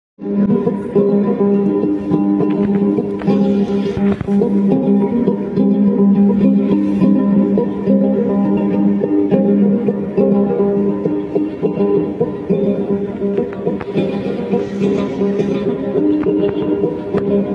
Traditional Music At Tjilik Riwut Airport Palangka Raya Central Kalimantan
Traditional Music at Tjilik Riwut Airport Palangka Raya Central Kalimantan | Musik Selamat Datang di Kalimantan Tengah